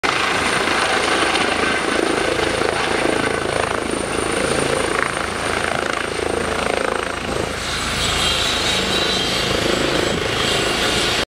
Sandige Landung | Christoph 34 Landung zum Wasserrettungs-Einsatz in M.V.
Zu sehen ist hier die Landung des Rettungshelikopter Christoph 34 von Wustrow - Ostseebad in M.V. nach einem Wasserrettungs-Einsatz, Abflug vom Sandstrand.

© BOS-BlaulichtPictures
Einsatzfahrzeuge im Fokus